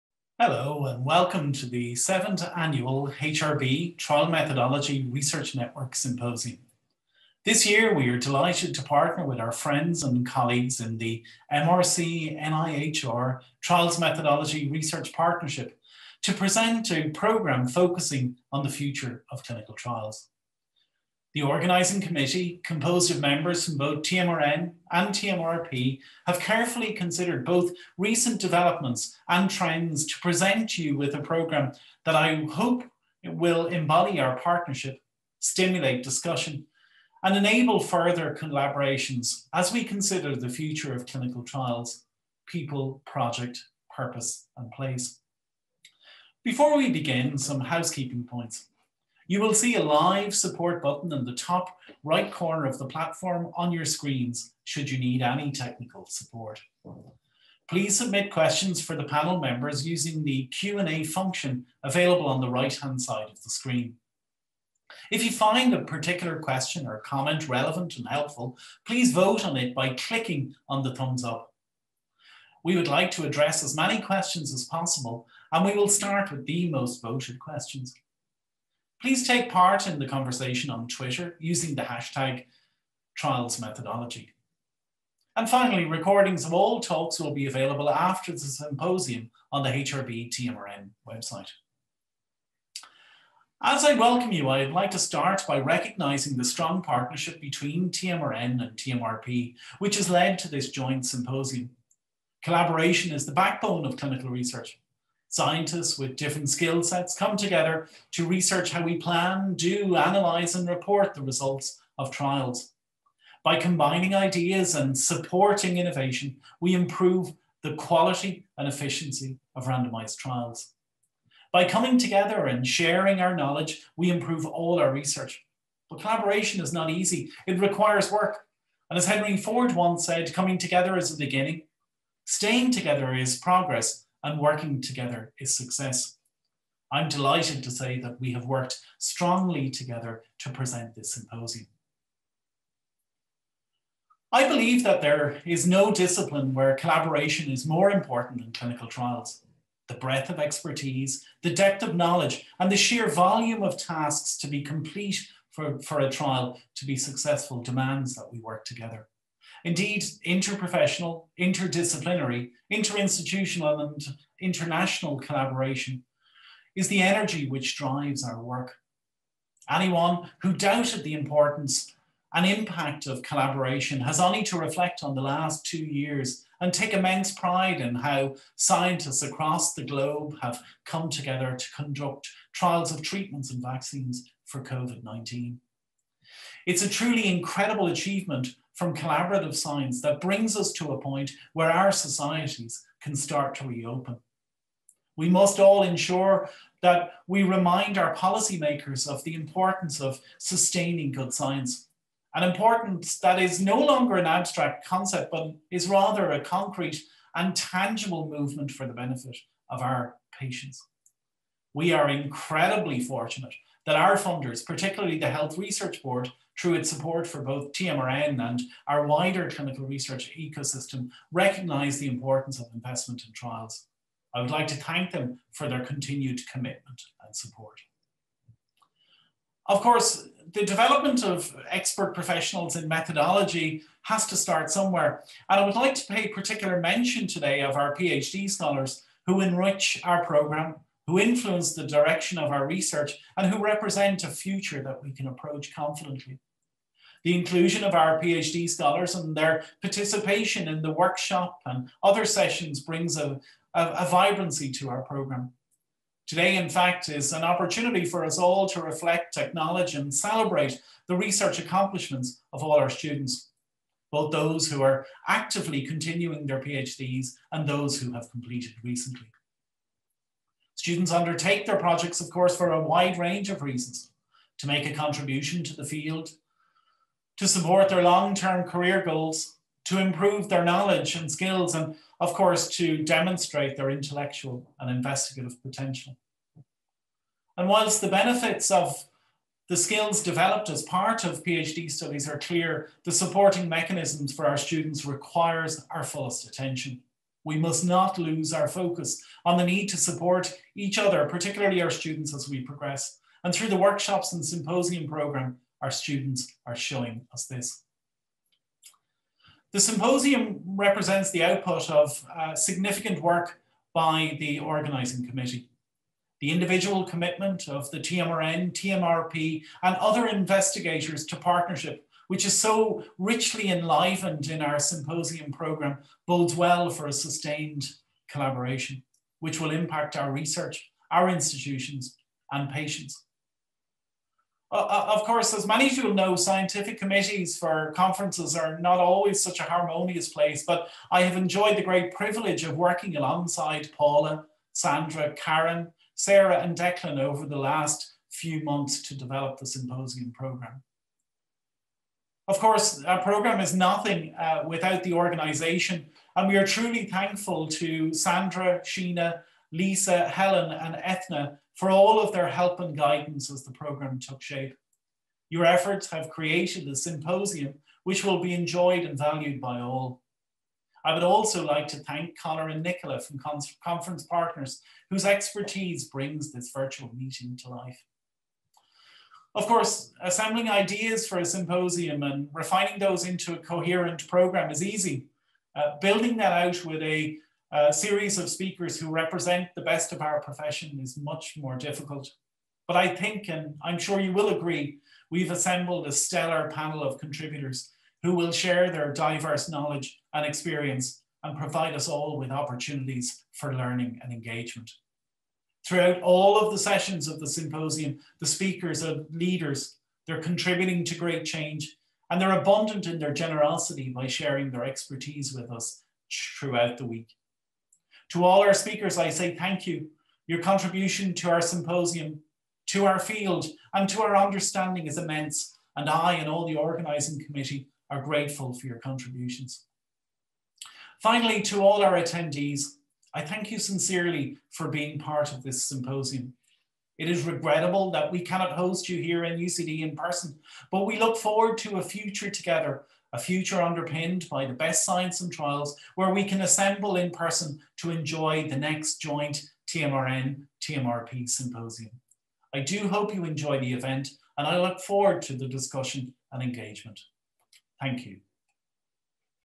7th Trials Methodology Symposium 2021 Prof Peter Doran Welcome Address
7th Trials Methodology Symposium Ireland & UK (Online)
The future of clinical trials: people, project, purpose, place
Hosted by HRB-TMRN (UCD) & MRC-NIHR-TMRP
October 2021
Prof Peter Doran, University College Dublin, Ireland
Welcome Address